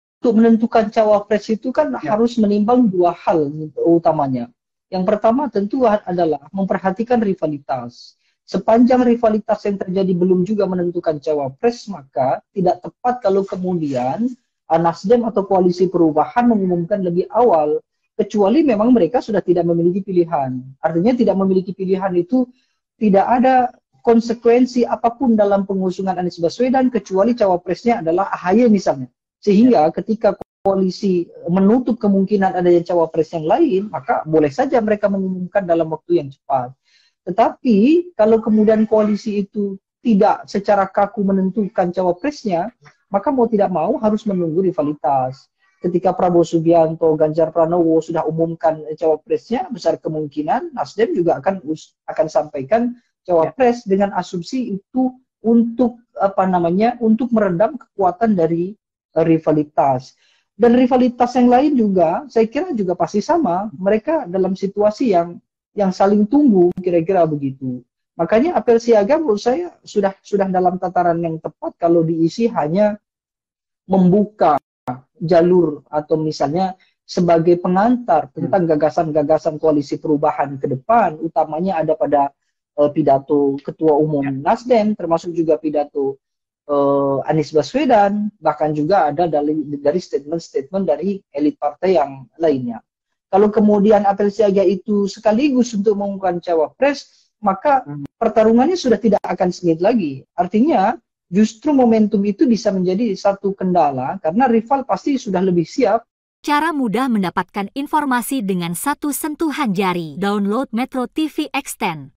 NASDEM TAK UMUMKAN CAWAPRES DI APEL SIAGA, INI KATA PENGAMAT
Partai NasDem telah menggelar Apel Siaga Perubahan (ASP) di Stadion Utama Gelora Bung Karno (SUGBK), Minggu (16/7). Dalam kegiatan tersebut, Partai NasDem belum mengumumkan calon wakil presiden (cawapres). Menurut Direktur Eksekutif IPO Dedi Kurnia Syah langkah NasDem sudah tepat. Hal tersebut tentu perlu mempertimbangkan rivalitas. Ia mengatakan apabila apel tersebut untuk mengumumkan cawapres, maka pertarungannya sudah tidak akan sengit lagi.